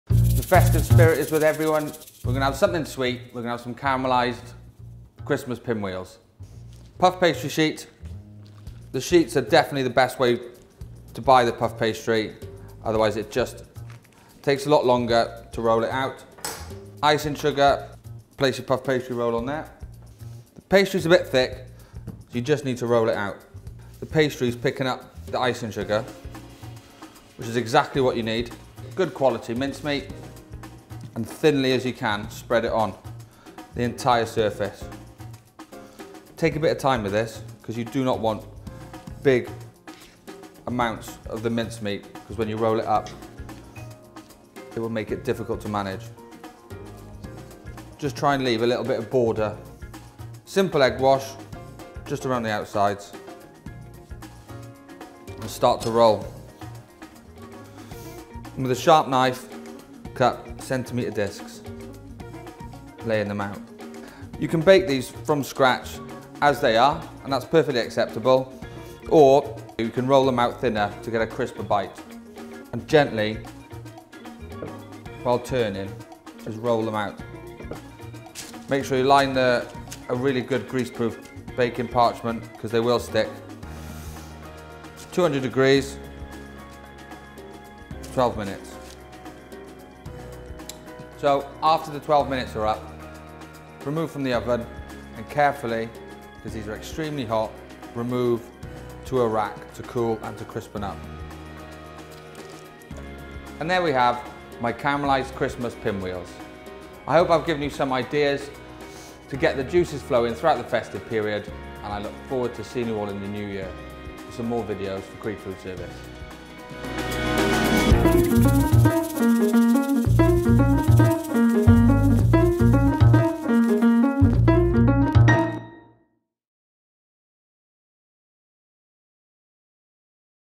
Caramelised Christmas Pinwheels
Perfect for Christmas parties and buffets, these pinwheels make a great alternative to the classic mince pie.